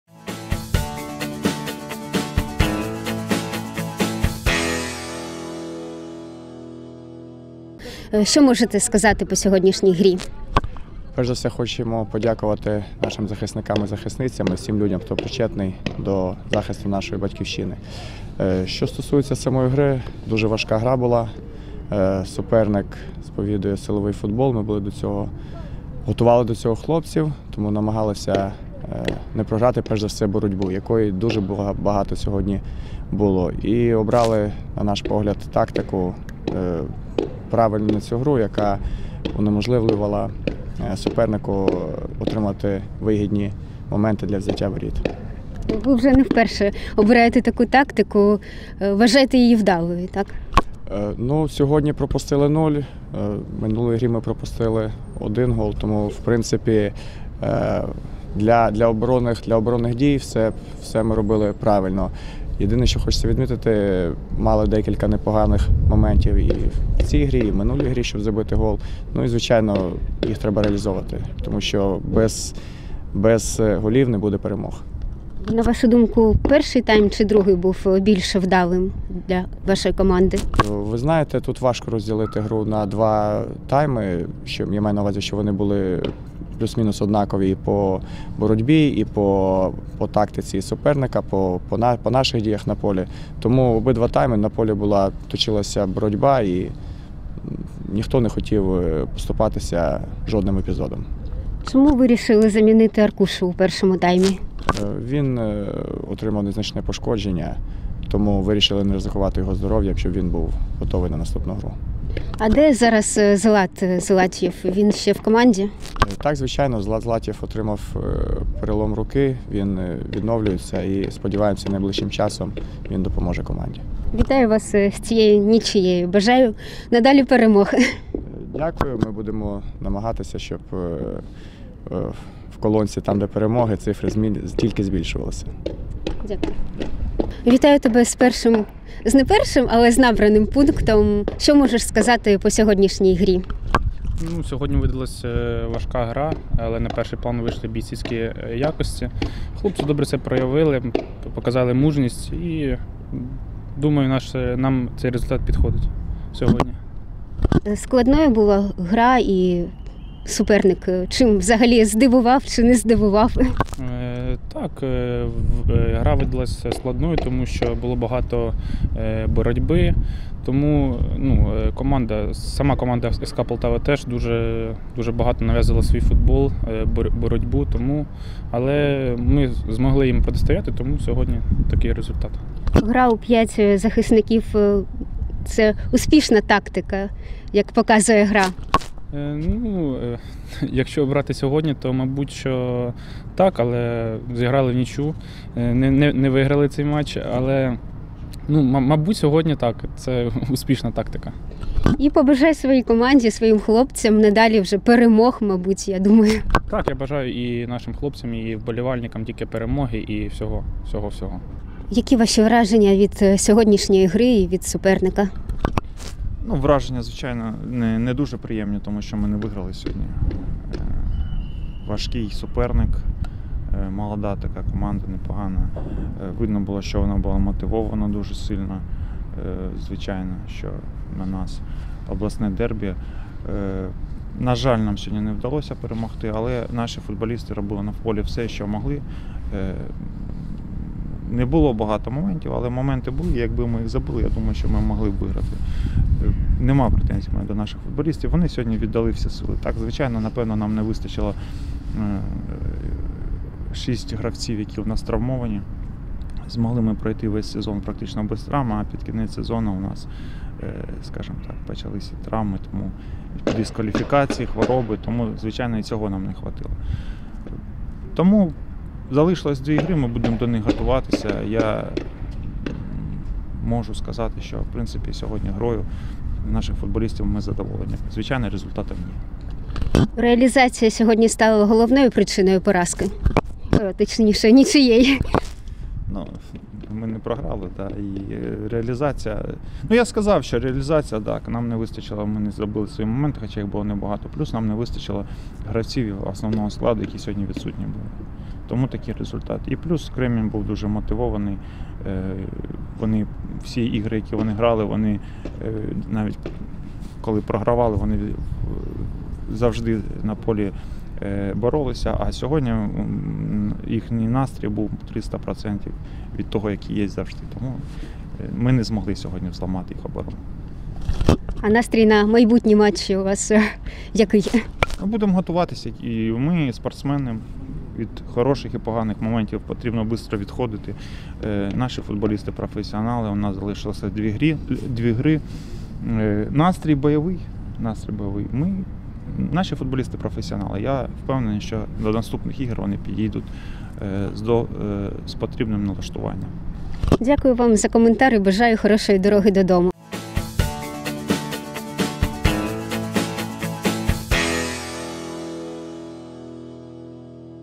Кремінь – Полтава | Коментарі матчу | Футбол | Група «Б» | Перша ліга ПФЛ
У матчі 16-го туру групи «Б» Першої ліги ПФЛ «Кремінь» та «Полтава» зіграли внічию. Поєдинок завершився з рахунком 0:0. До вашої уваги післяматчеві коментарі.

DiviSport - офіційний мовник Першої ліги Професіональної футбольної ліги України сезону 2024/25!